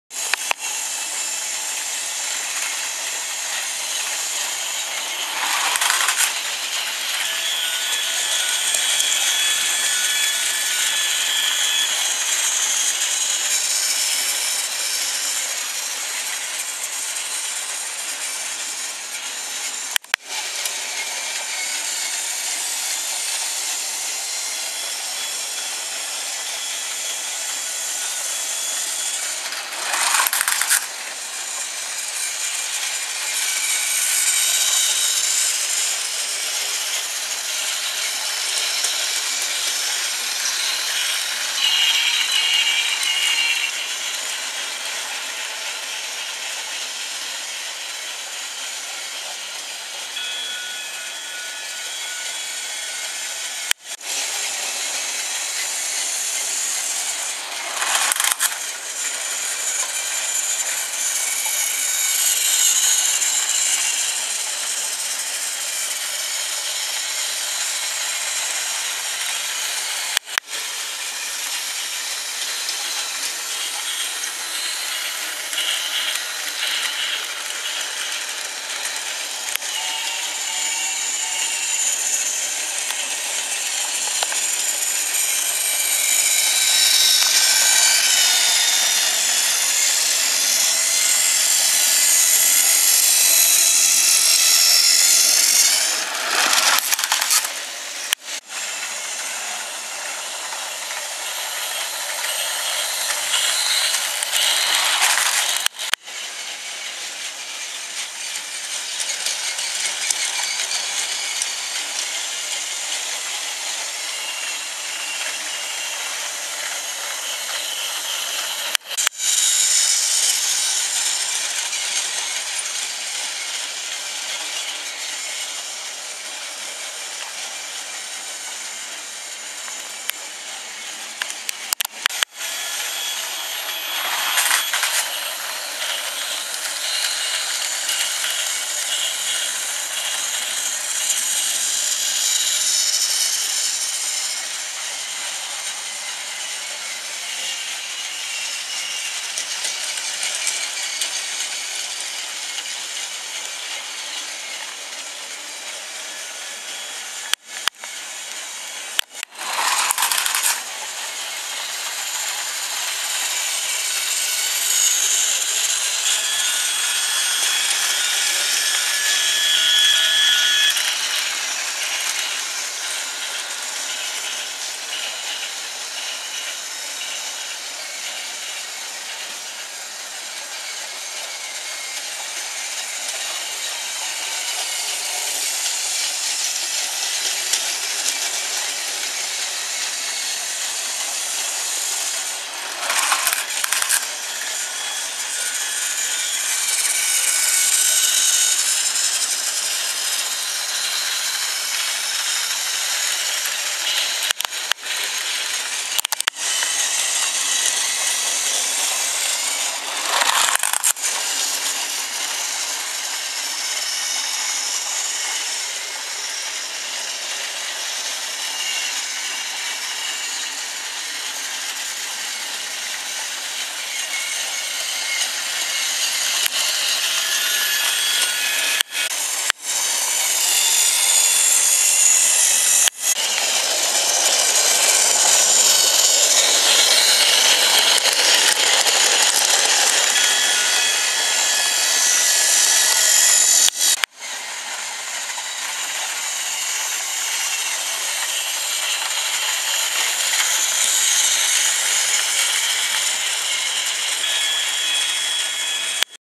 Video for Children Toy Trains Animal Rail Safari 2 for Kiddies Videos
Ho hey guys, big Duncan giving more zoo animals a trip around Trackmaster rails.
(Copyright vxbatabs 2014)